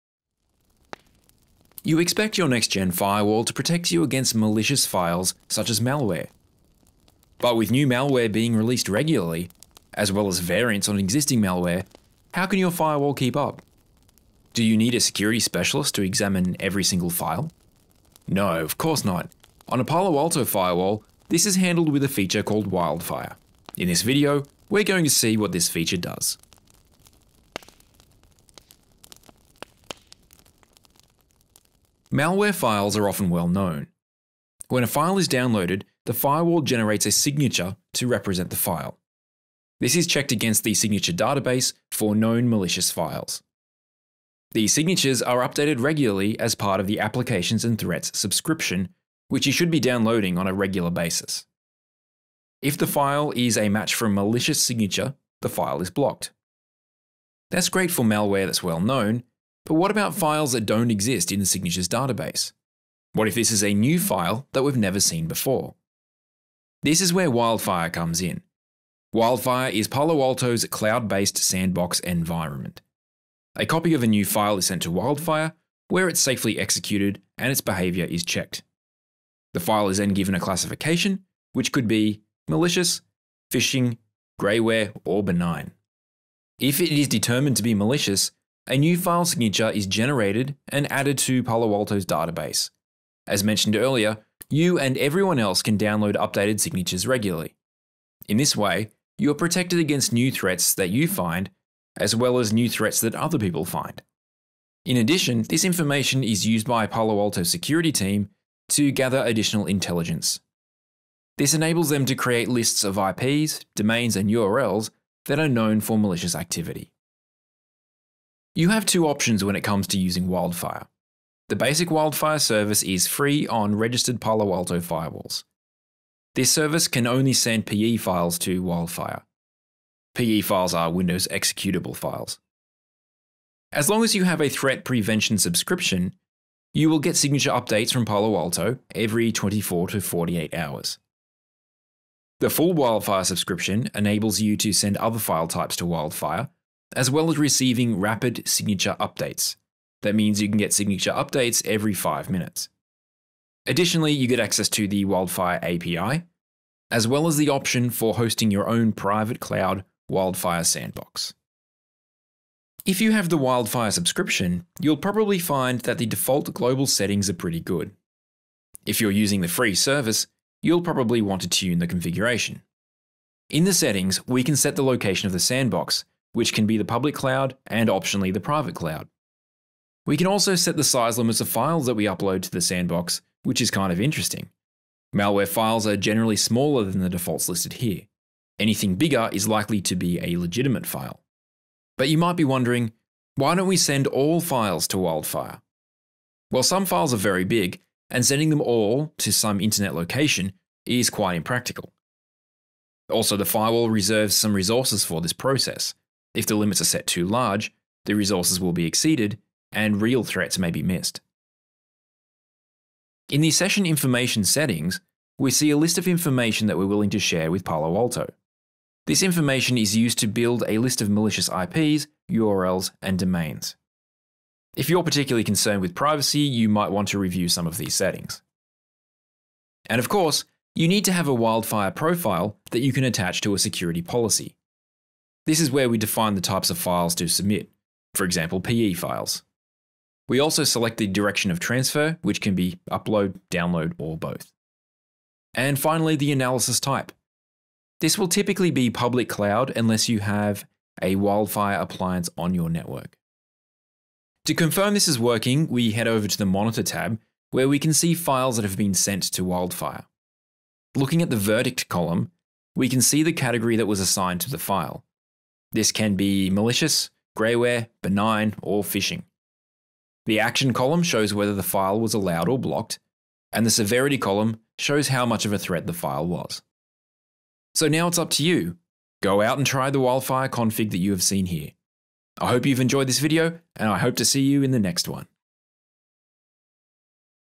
WildFire | Palo Alto Firewall Training
Wildfire is Palo Alto’s solution to analysing new files, to determine if they are a threat. Of course, the firewall will be able to drop known malware, but how will it handle unknown files?

When an unknown file comes in, the firewall forwards it to the Wildfire cloud environment. This is an isolated area where the file is analyzed, to see if it is a threat. It is then given a posture assessment; malicious, grayware, phishing, or benign.

When new threats are found, they are added to the signature database, which we download regularly. If there are several threats from a particular IP or URL, these may also be marked as a threat.

In this way, we all help to keep each other safe.

Overview of this video:

0:00 Introduction

0:30 Wildfire

1:57 Licensing

2:41 Configuration

4:12 Verification